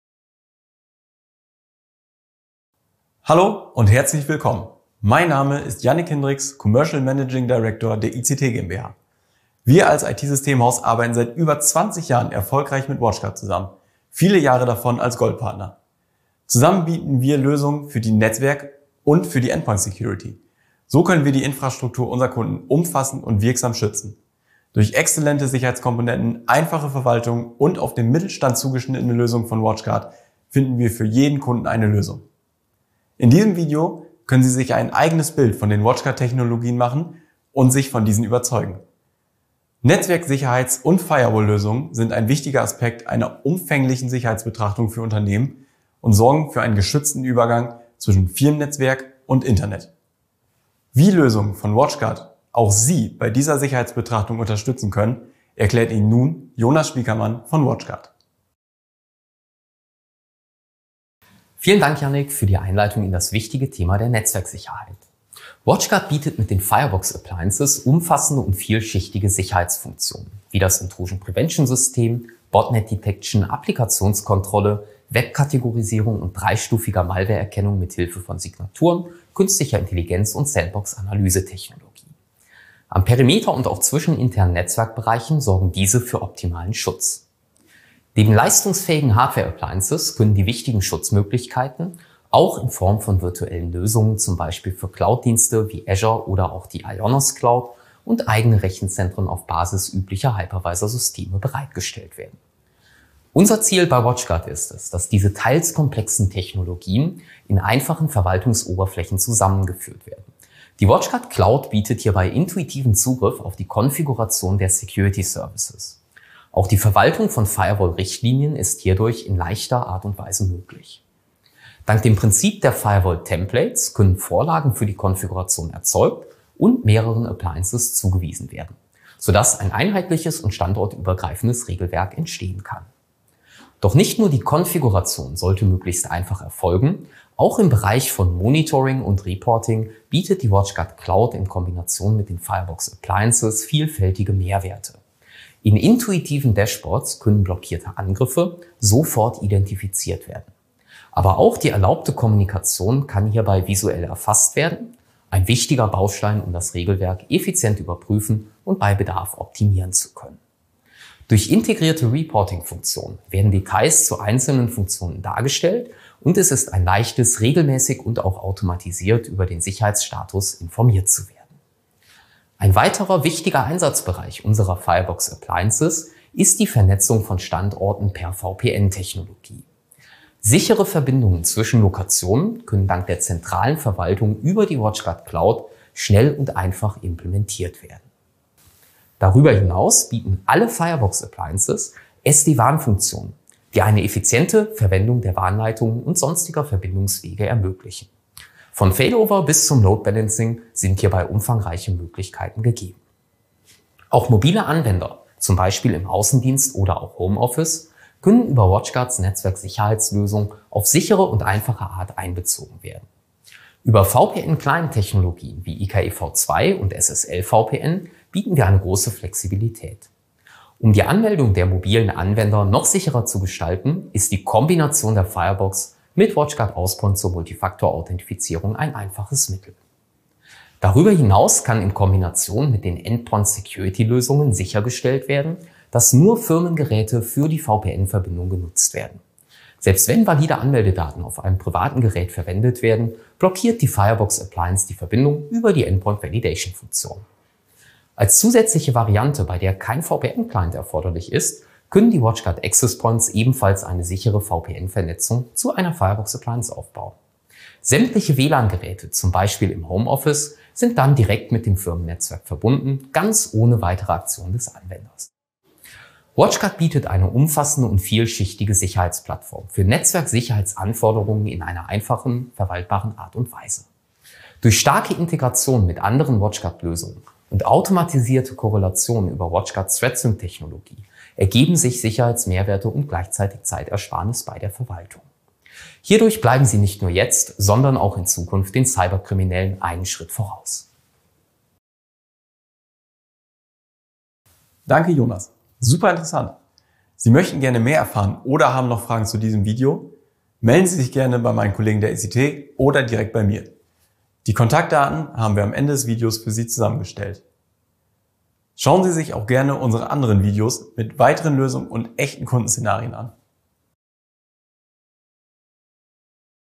ICT & Watchguard - Firebox
Wir bei der ICT GmbH arbeiten seit über 20 Jahren mit WatchGuard zusammen, natürlich auch im Firewall Bereich. In diesem Video erklärt Ihnen Jonas Spieckermann von WatchGuard zusammen mit Jannik Hindriks von der ICT die Einrichtung und Funktionsweise der Fireboxen und zeigt Ihnen die Mehrwerte für Ihr Unternehmen.
Ein spannender Einblick in die Arbeit von WatchGuard, die wir Ihnen gerne zeigen.
Die Kontaktdaten der ICT GmbH finden Sie am Ende des Videos.